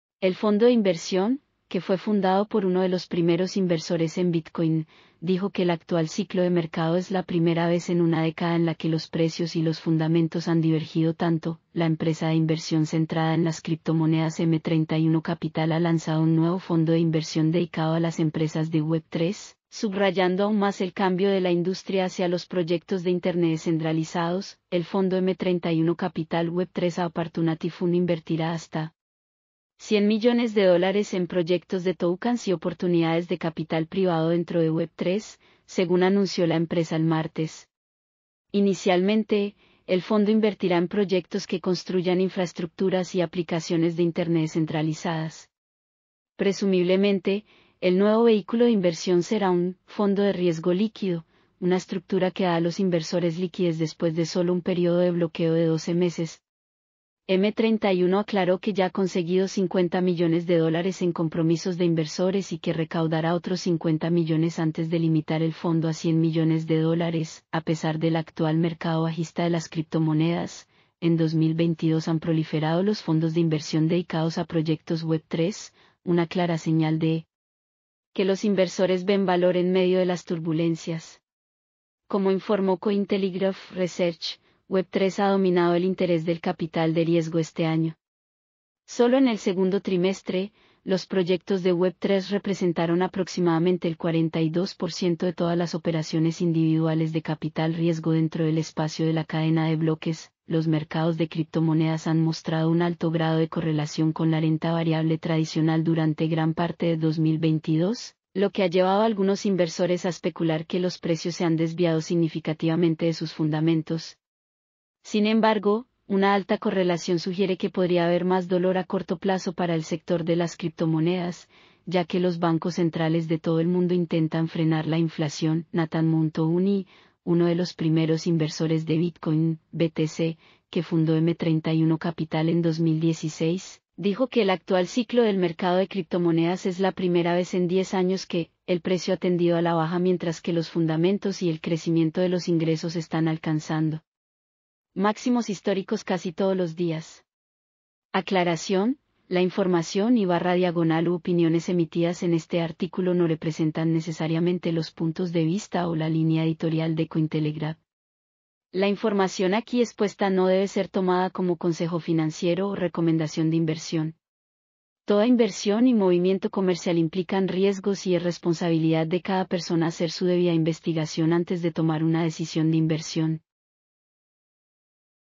🥇 M31 Capital lanza un fondo de inversión de 100 millones de dólares en Web3 con 50 millones de dó
El fondo de inversión, que fue fundado por uno de los primeros inversores en Bitcoin, dijo que el actual ciclo de mercado es la primera vez en una década en la que los precios y los fundamentos han divergido tanto